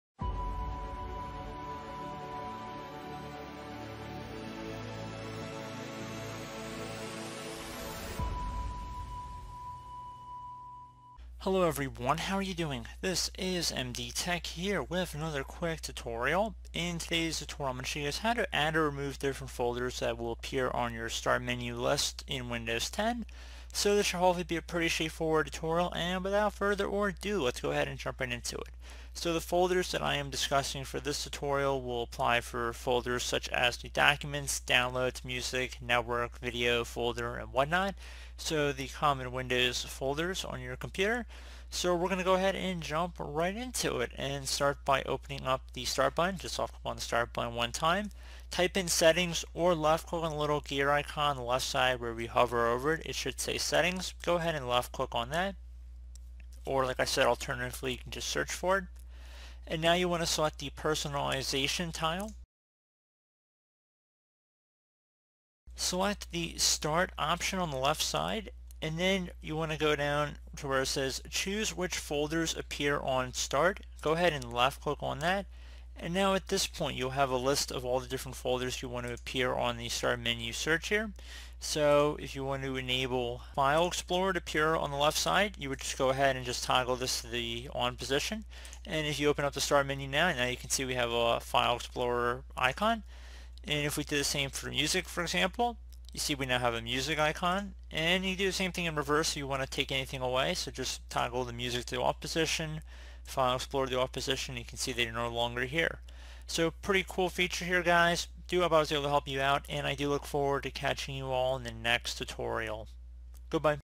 How to Add or Remove Folders on Start List in Windows 10 [Tutorial]
The Start menu in Windows 10 has a Start list that you can customize which folders appear in the list. You can add or remove the File Explorer, Settings, your Documents, your Downloads, your Music, your Pictures, your Videos, Network, and Personal folder to your Start list to open from your Start menu.

Some of these folders (ex: File Explorer) will display a Jump List (expandable menu caret) for items you recently opened with the link.

Starting with Windows 10 build 14328, any additional folders you have chosen to appear on Start will now be immediately available on Start and you will no longer need to open the hamburger button to access them.

This tutorial will show you how to choose which folders appear on Start list for your account in Windows 10.